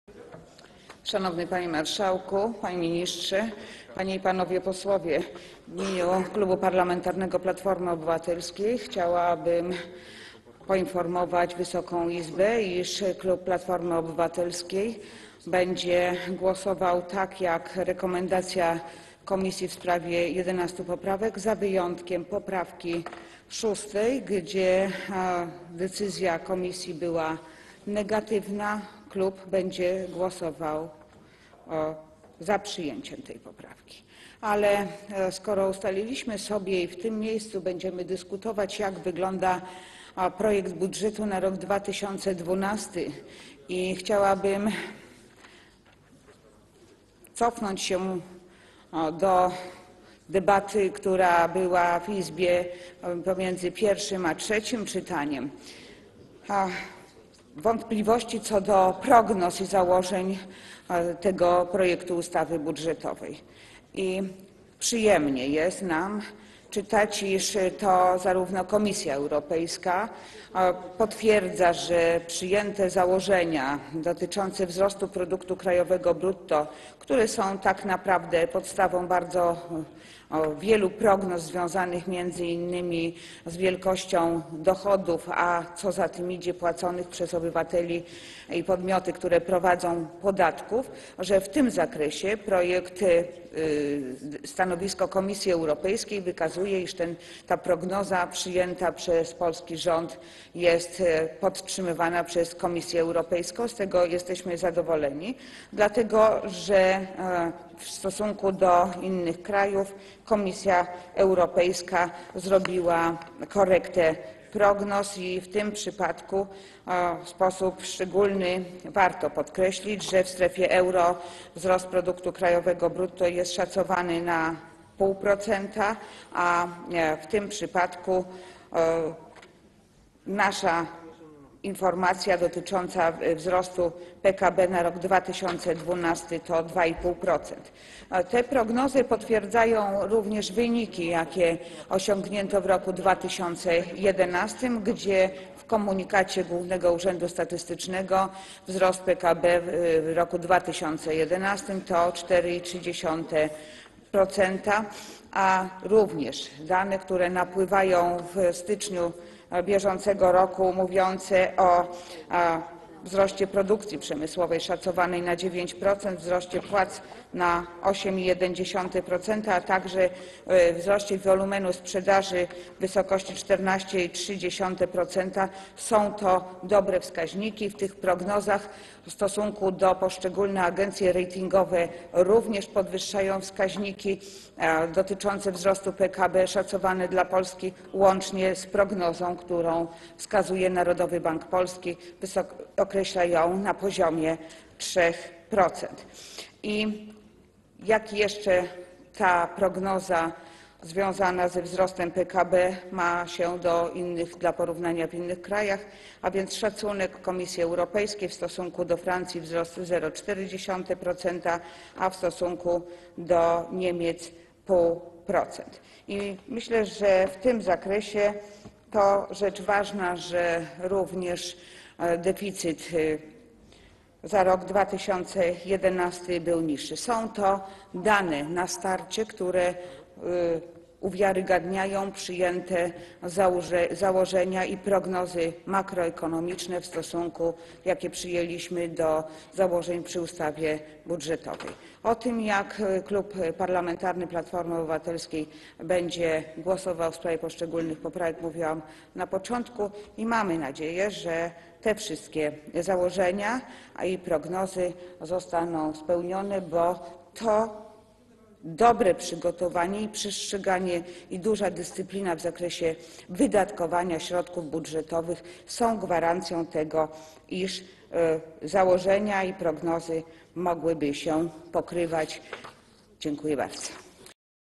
[299/376] Krystyna Skowrońska: Szanowny Panie Marszałku! Panie Ministrze! Panie i Panowie Posł...
Posiedzenie Sejmu nr 9
1 marca 2012 r. 18:18:56
Poseł Krystyna Skowrońska:

Szanowny Panie Marszałku!  Panie Ministrze!  Panie i Panowie Posłowie!  W imieniu Klubu Parlamentarnego Platforma Obywatelska chciałabym poinformować Wysoką Izbę, iż klub Platforma Obywatelska będzie głosował zgodnie z rekomendacją komisji w sprawie 11 poprawek, z wyjątkiem poprawki 6., w przypadku której decyzja komisji była negatywna, a klub będzie głosował za jej przyjęciem.  Skoro ustaliliśmy i w tym miejscu będziemy dyskutować, jak wygląda projekt budżetu na rok 2012, chciałabym cofnąć się do debaty, która odbyła się w Izbie pomiędzy pierwszym a trzecim czytaniem.

Chodzi o wątpliwości co do prognoz i założeń tego projektu ustawy budżetowej.  Przyjemnie jest nam czytać, iż Komisja Europejska potwierdza, że przyjęte założenia dotyczące wzrostu produktu krajowego brutto, które są tak naprawdę podstawą bardzo wielu prognoz związanych między innymi z wielkością dochodów, a co za tym idzie, płaconych przez obywateli i podmioty, które prowadzą działalność, podatków są słuszne.  W tym zakresie Komisja Europejska w swoim stanowisku podtrzymuje przyjętą przez polski rząd prognozę.  Z tego jesteśmy zadowoleni, dlatego że w stosunku do innych krajów Komisja Europejska zrobiła korektę prognoz.  W sposób szczególny warto podkreślić, że w streﬁe euro wzrost produktu krajowego brutto jest szacowany na 0,5%, a według informacji wzrost naszego PKB na rok 2012 to 2,5%.

Te prognozy potwierdzają również wyniki, jakie osiągnięto w roku 2011, bo zgodnie z komunikatem Głównego Urzędu Statystycznego wzrost PKB w roku 2011 to 4,3%, jak też dane, które napłynęły w styczniu bieżącego roku, wskazujące na wzrost produkcji przemysłowej szacowany na 9%, wzrost płac w wysokości 8,1%, a także wzrost wolumenu sprzedaży w wysokości 14,3%.  Są to dobre wskaźniki, te ujęte w tych prognozach.  Poszczególne agencje ratingowe również podwyższają wskaźniki dotyczące szacowanego wzrostu PKB dla Polski.  Łączy się to z prognozą, którą przedstawia Narodowy Bank Polski, który określa ten wzrost na poziomie 3%.  I jak jeszcze ta prognoza wzrostu PKB dla porównania ma się do prognozy w innych krajach?

A więc szacunki Komisji Europejskiej w stosunku do Francji to wzrost w wysokości 0,4%, a w stosunku do Niemiec -- 0,5%.  Myślę, że w tym zakresie to rzecz ważna, że również deﬁcyt za rok 2011 był niższy.  Są to dane, które na starcie uwiarygodniają założenia i prognozy makroekonomiczne, jakie przyjęliśmy do ustawy budżetowej.  O tym, jak Klub Parlamentarny Platforma Obywatelska będzie głosował w sprawie poszczególnych poprawek, mówiłam na początku.  Mamy nadzieję, że te wszystkie założenia i prognozy się spełnią, bo dobre przygotowanie i duża dyscyplina w zakresie wydatkowania środków budżetowych są gwarancją tego, iż założenia i prognozy mogłyby się pokrywać z realizacją.

Dziękuję bardzo.

(Oklaski)